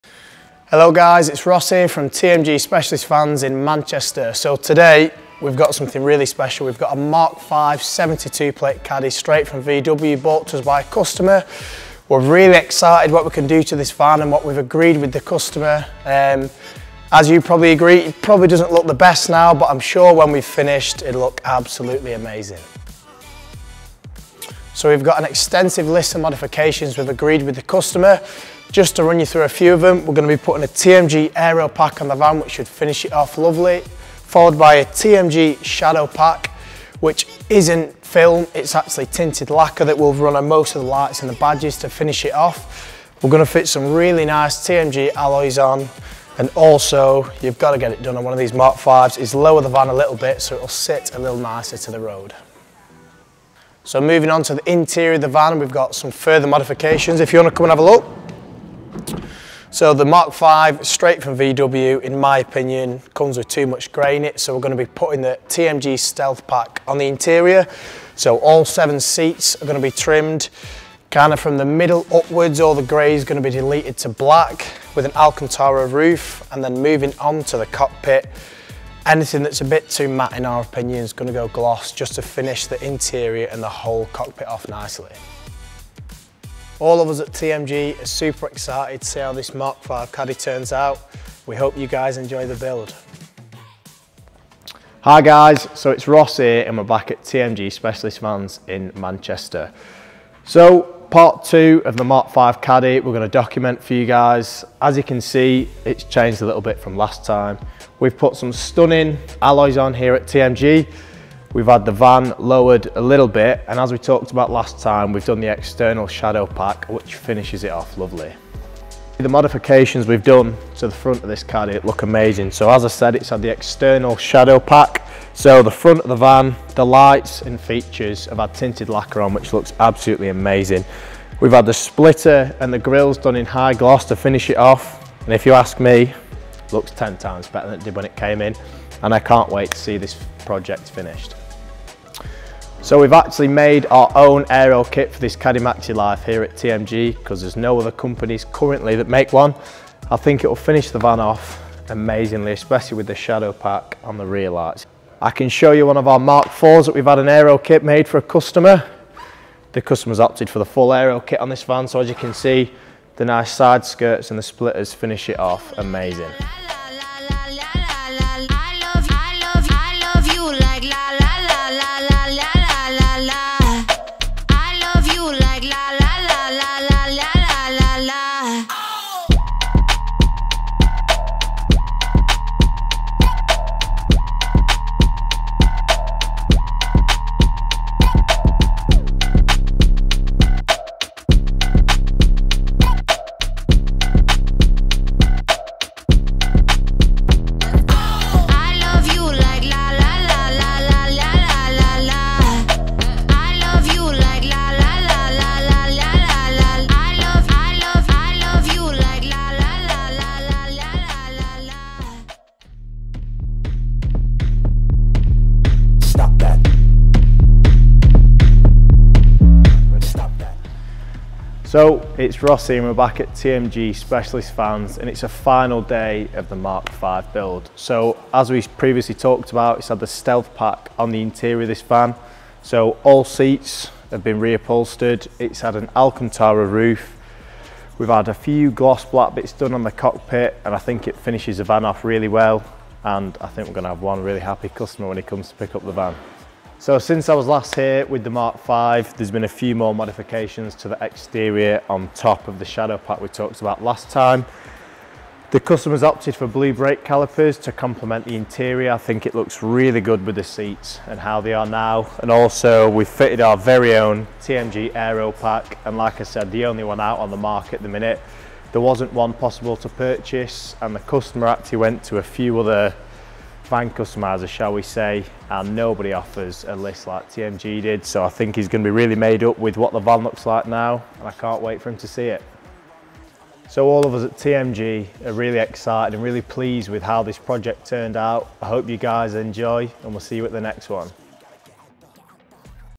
TMG SPECIALIST VANS | VW CADDY 5 CUSTOMER CUSTOMIZATION | CUSTOM CADDY
VW Caddy MK5 customized at TMG. We cover it all under one roof!